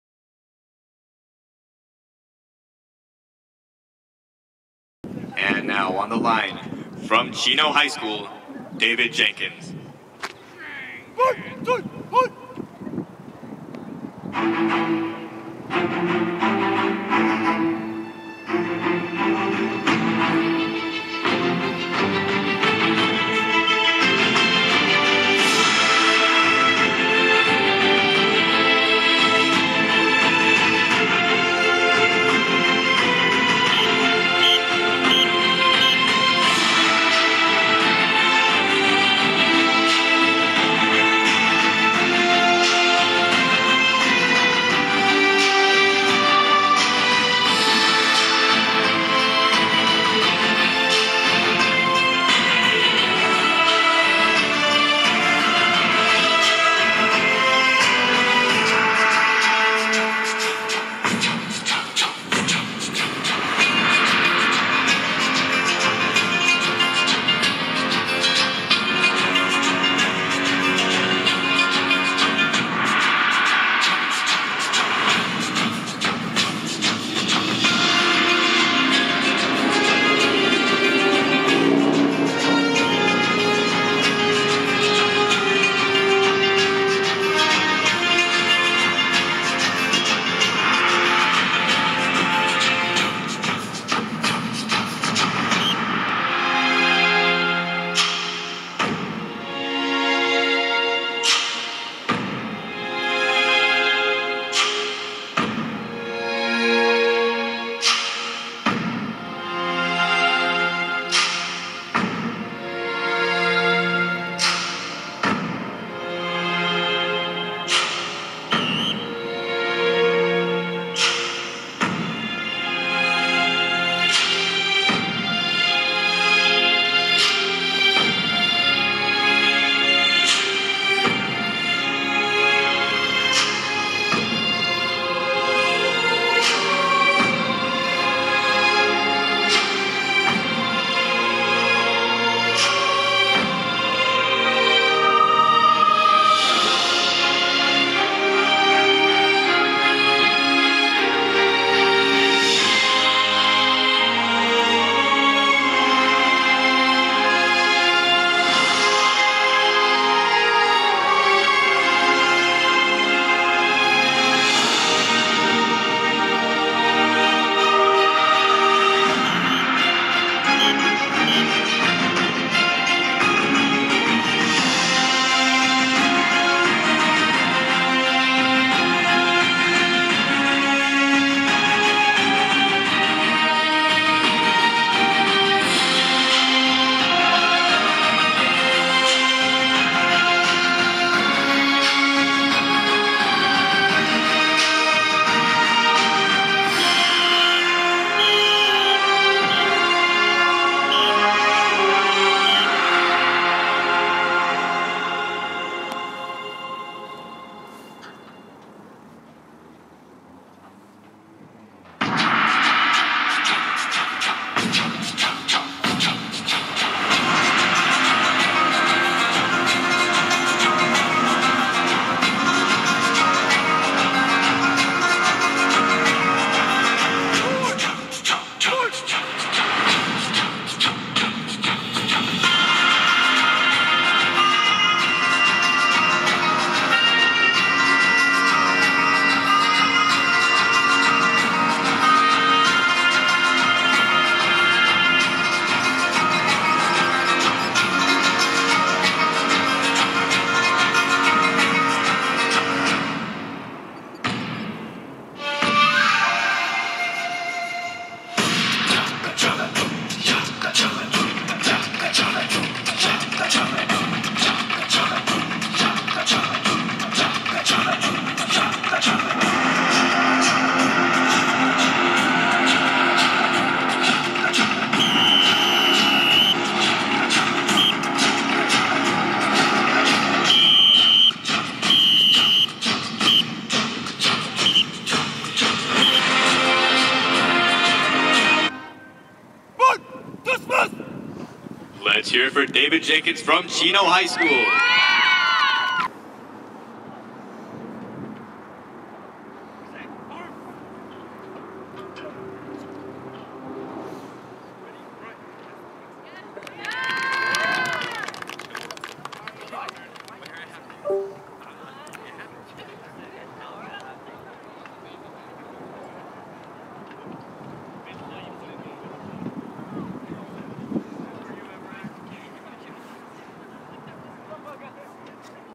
David Jenkins Avante Garde representing Chino High School at Rancho Verde
Avant Garde, 1st Place, 91.45

Penalty: -0

Senior